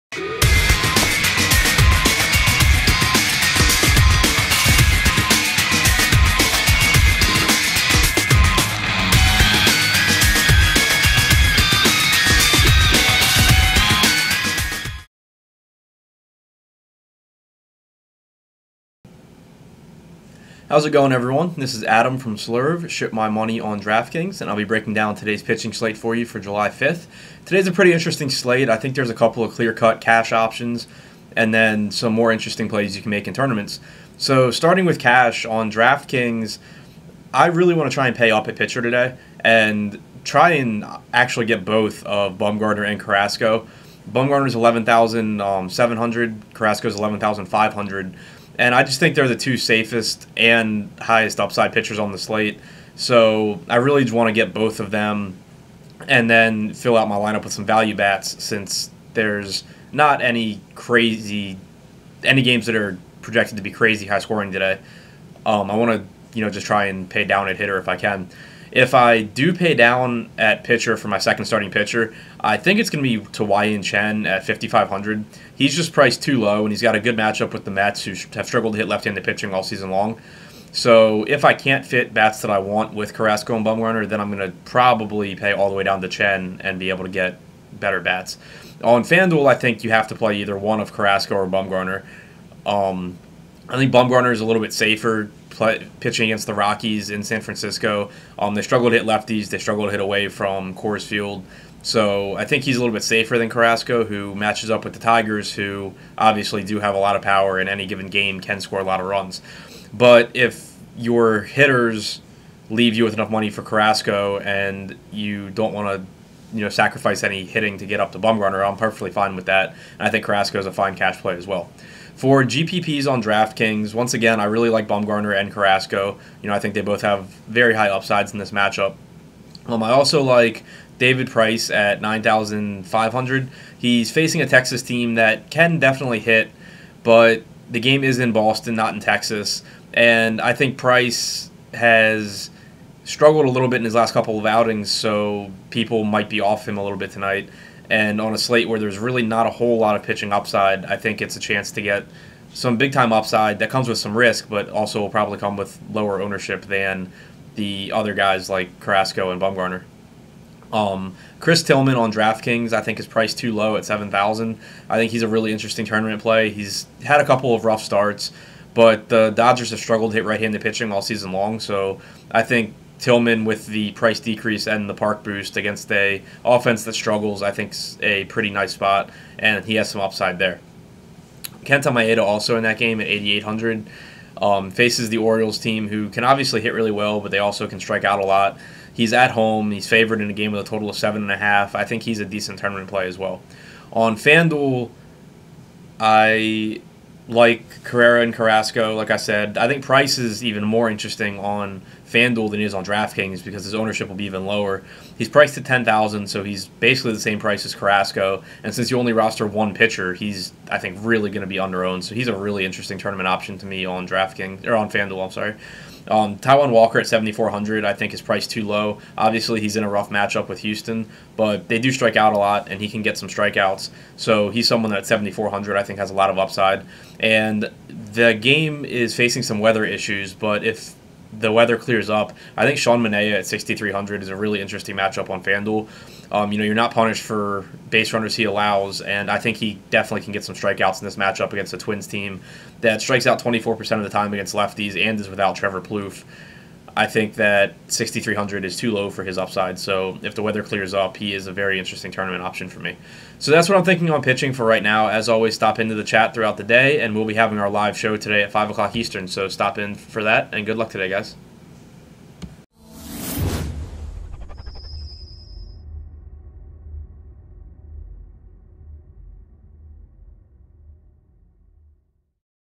MLB Pitching Analysis - 7/5
Adam Scherer (@shipmymoney) discusses the best pitching options for tonight's MLB slate. Chat with all of our experts in the Tailgate